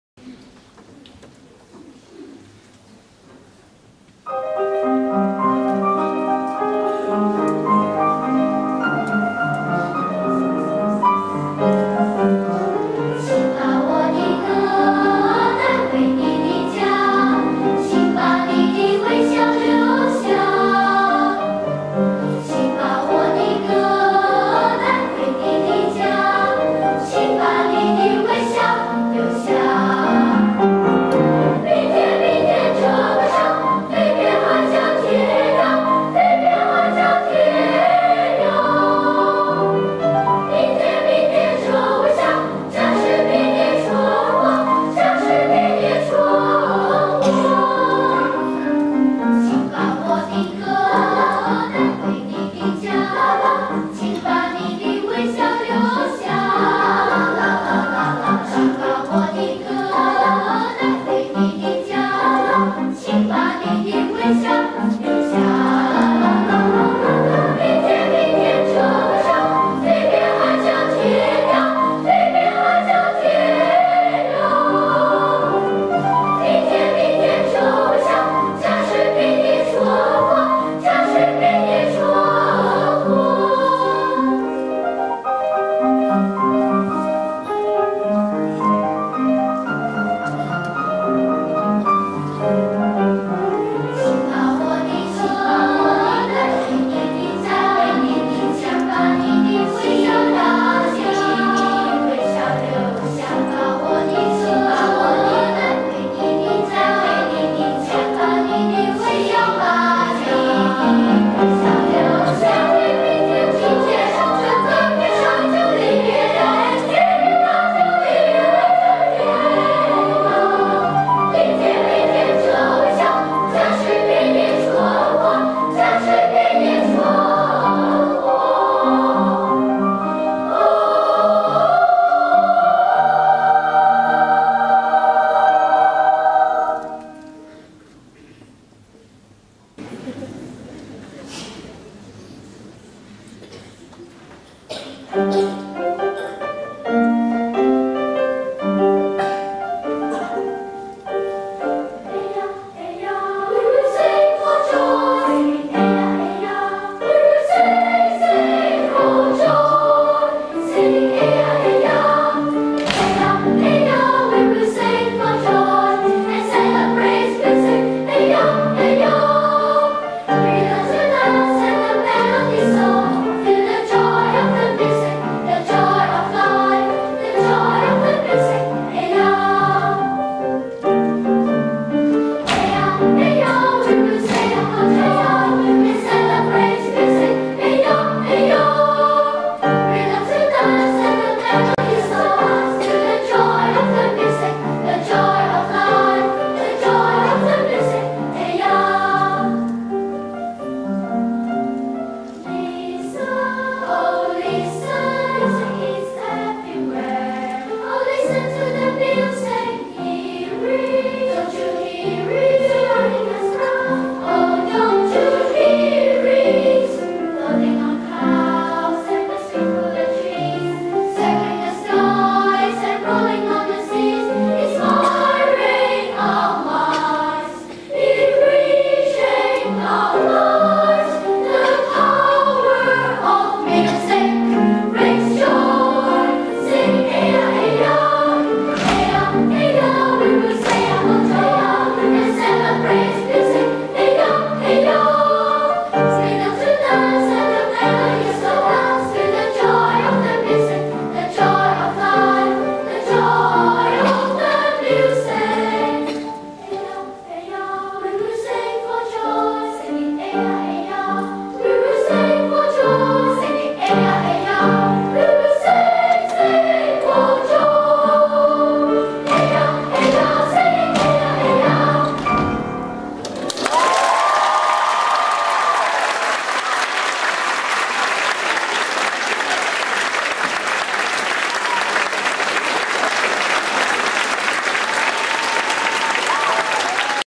Yuen Long Merchants Association Primary School Choir - Hong Kong
Conductor: Ms Helen Lam Hoi Yan

ASIA CANTATE INTERNATIONAL CHORAL FESTIVAL 2016, HONG KONG

Category Winner, Children's Choir
Grand Finalist